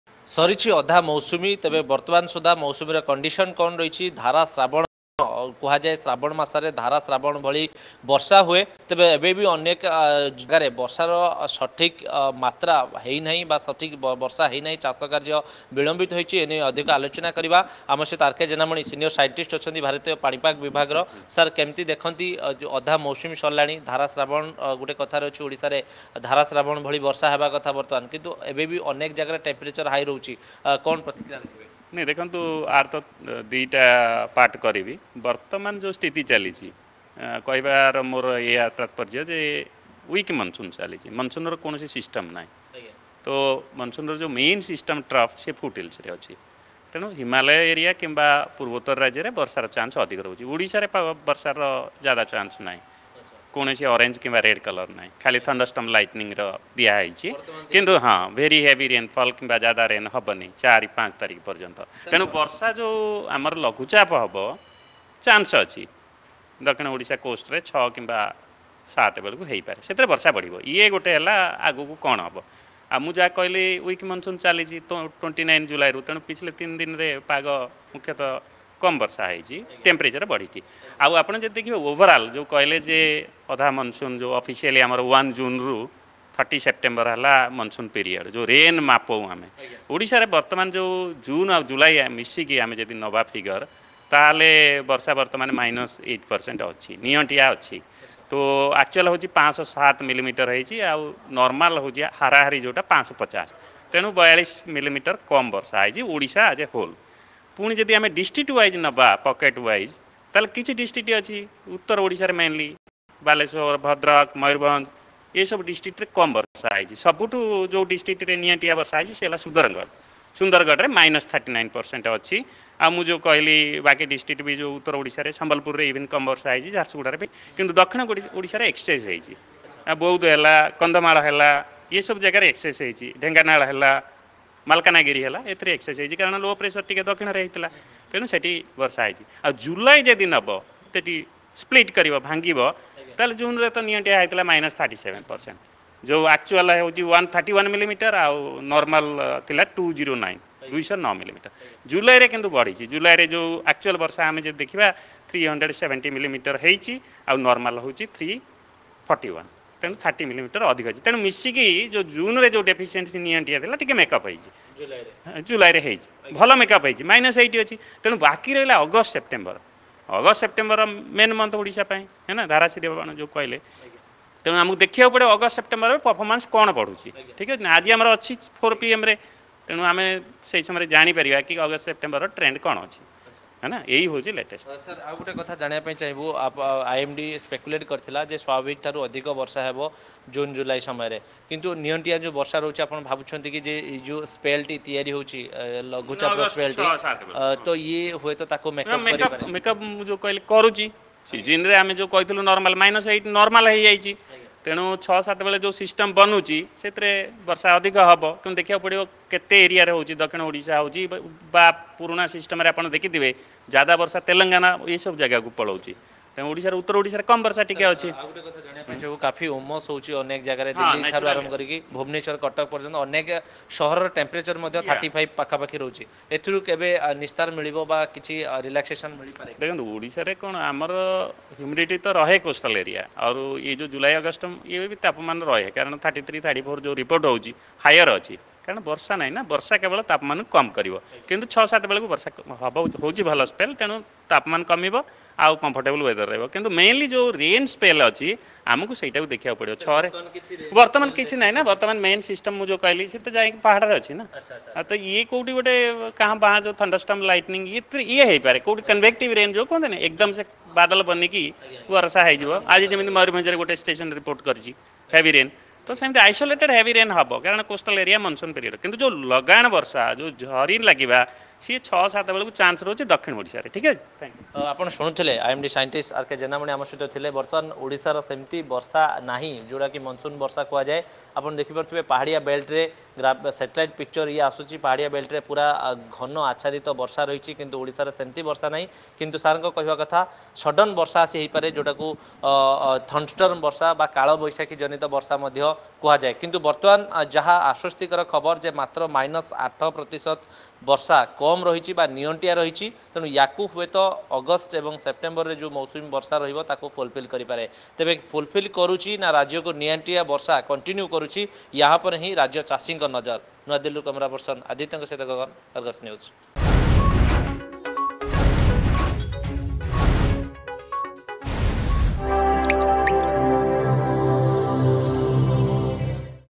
ଶ୍ରାବଣରେ ହଜି ଯାଇଛି ବର୍ଷା ଏ ନେଇ ଅଧିକ ସୂଚନା ଦେଇଛନ୍ତି
IMD ର ବରିଷ୍ଠ ବୈଜ୍ଞାନିକ ରାଜେନ୍ଦ୍ର ଜେନାମଣିଙ୍କ ସୂଚନା ।